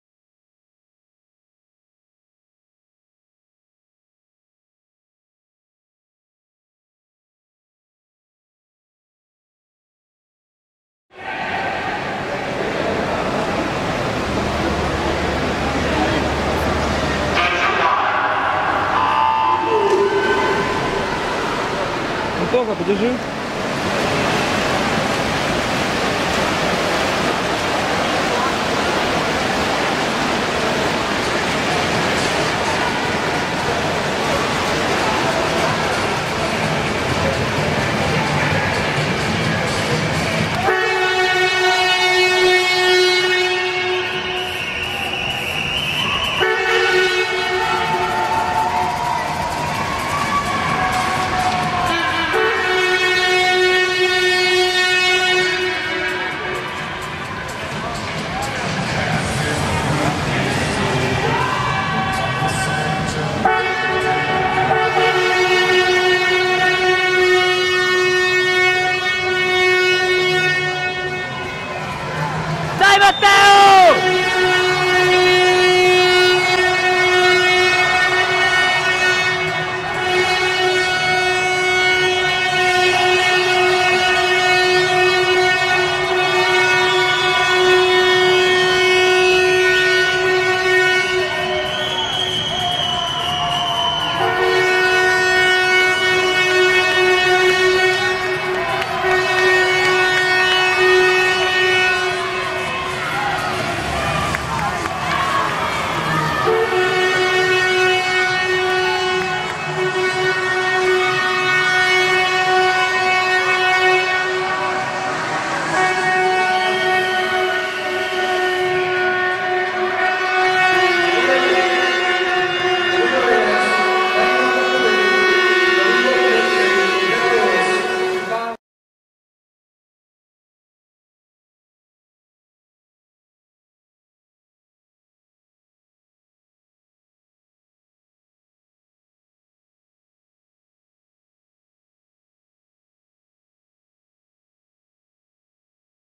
200 bf Men Finswimming European Junior Championship 2009
12th CMAS Finswimming European Junior Championship Belgrade (Serbia) 12/19 July 2009 Final 200 bf Men